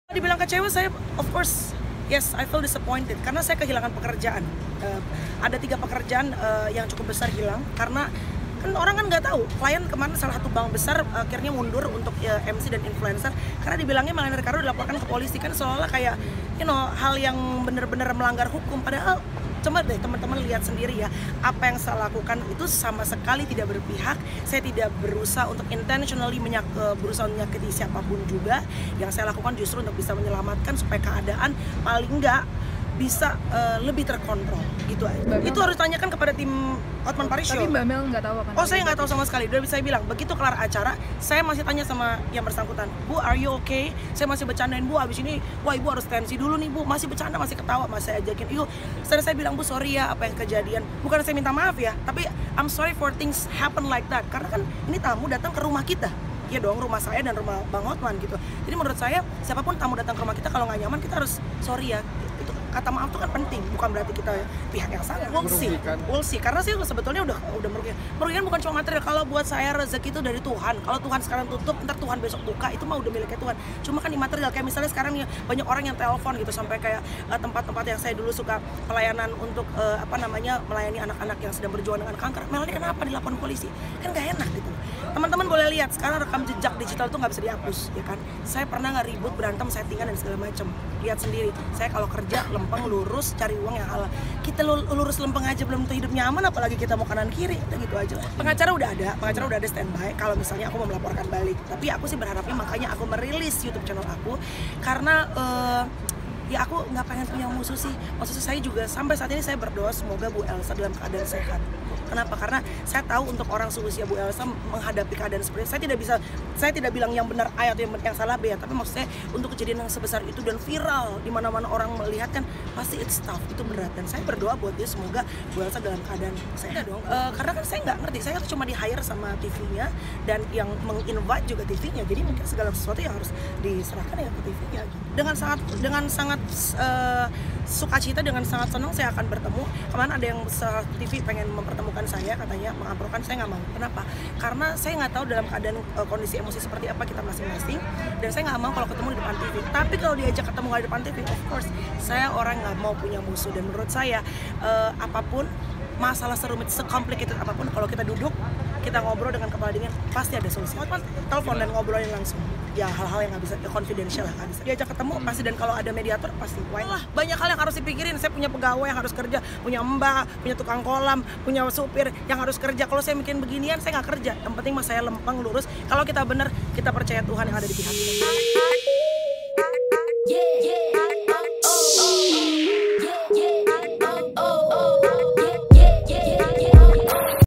Meski Kehilangan 3 Job, setelah Kabar dilaporkan, Melanie Ricardo Tetep Mendoakan Elsa Syarif
Meski Kehilangan 3 Job, setelah Kabar dilaporkan, Melanie Ricardo Tetep Mendoakan Elsa Syarif Semoga  Sehat